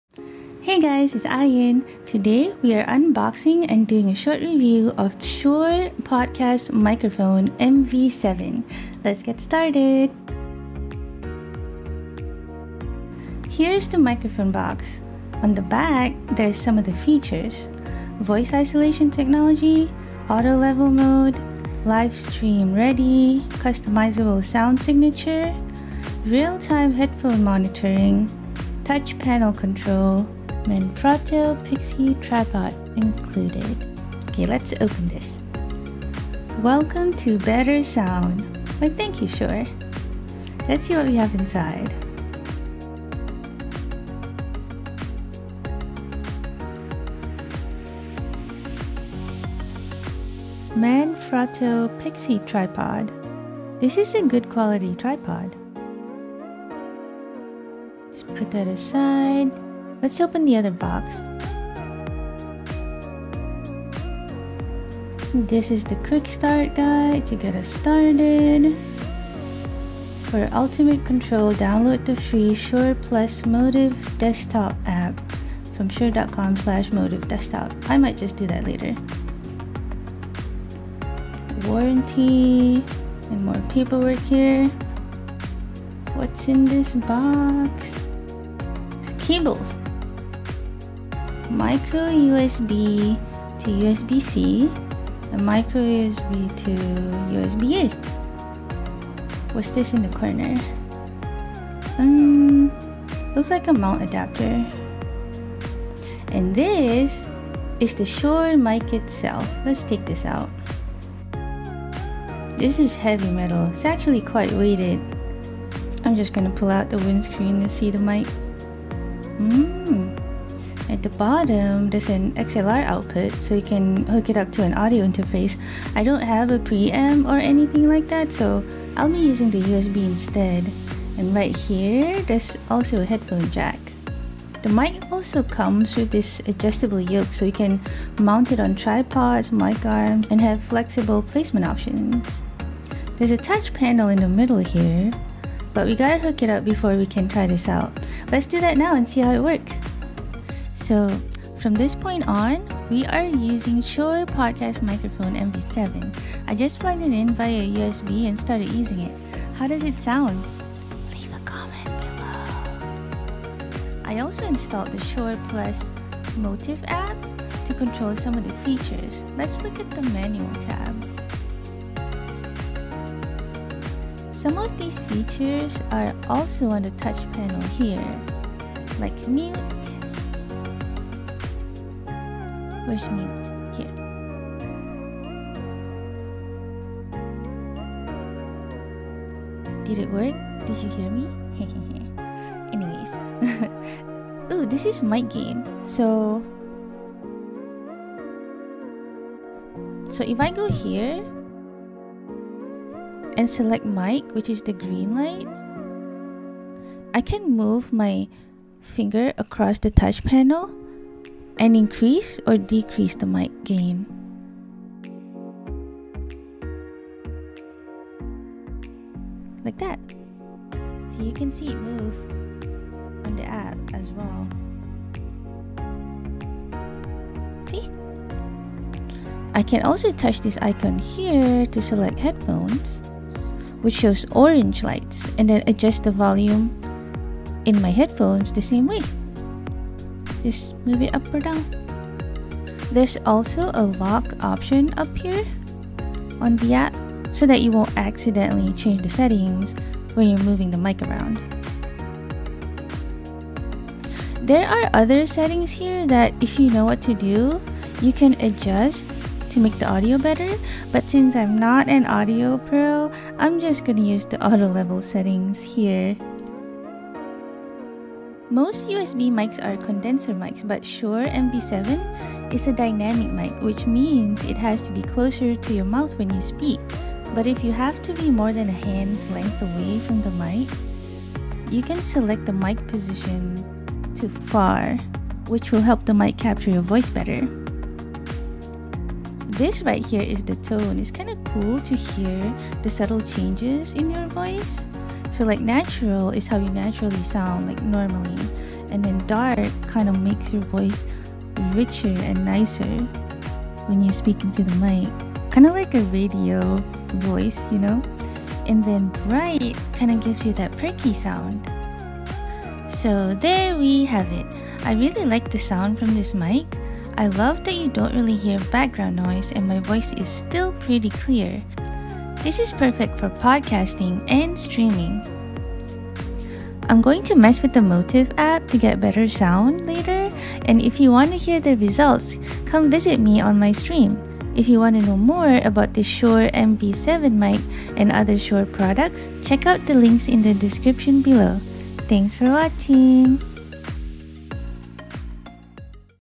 Shure Podcast Microphone MV7 Unboxing & Review
►Timestamp
0:00 Intro
0:10 MV7 Unboxing
1:45 Shure MV7
2:30 Now using Shure MV7!
2:46 ShurePlus Motiv app
5:12 Final thoughts

Inspired by the legendary SM7B, the MV7 is a dynamic microphone with both USB
and XLR outputs for use with computers and professional interfaces alike.
Connect via USB and explore additional set-up features and Auto Level Mode
within the ShurePlus™ MOTIV app, to control your vocal tone, and distance from
the microphone.